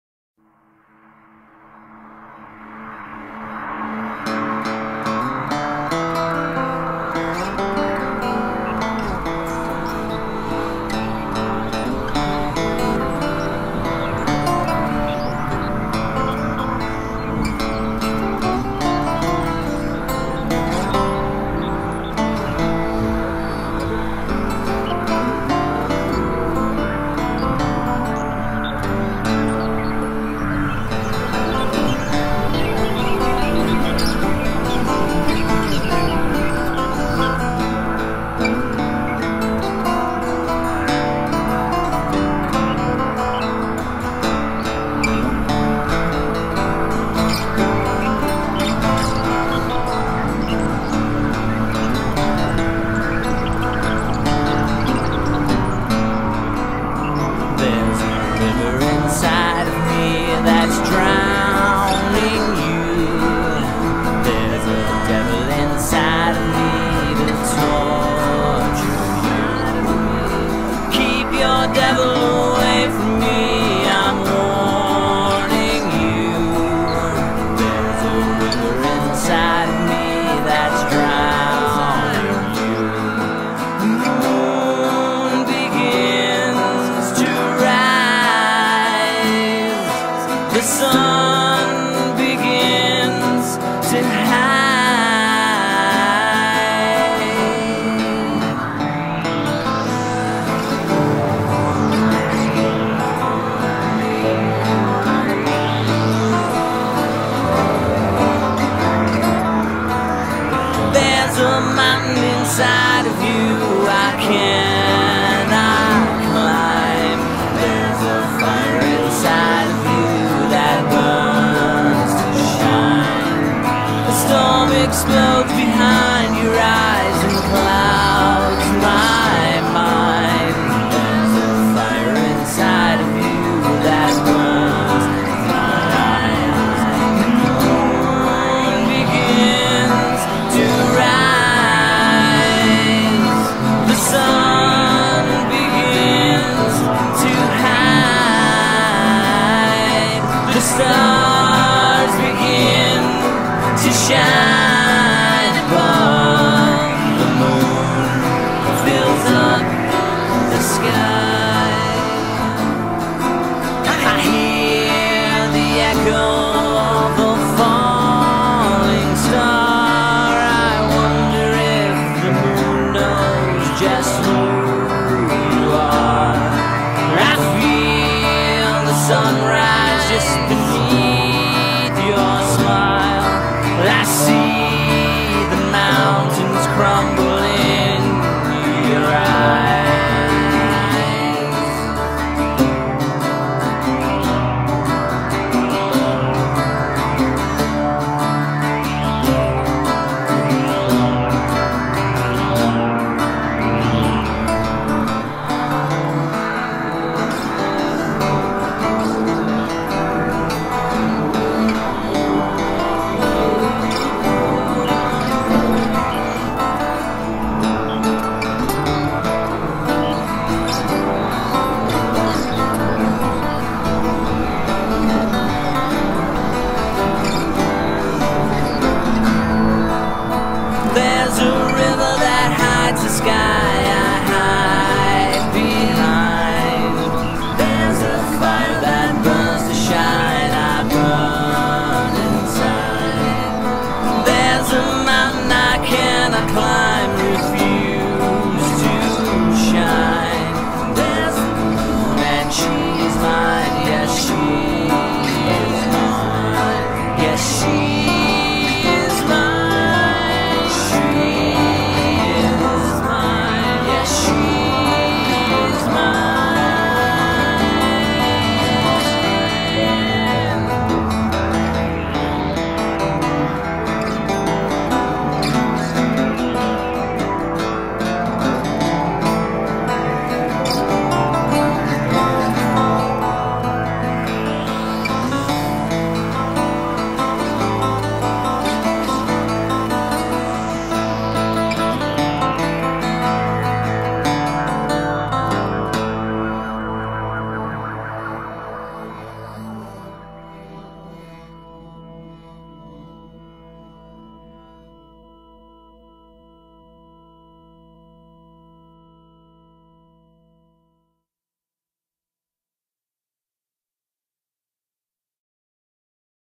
Keep Your Devil.m4v
more randomness. video from the past 10 years fused in to images from the past week in Manhattan and Brooklyn...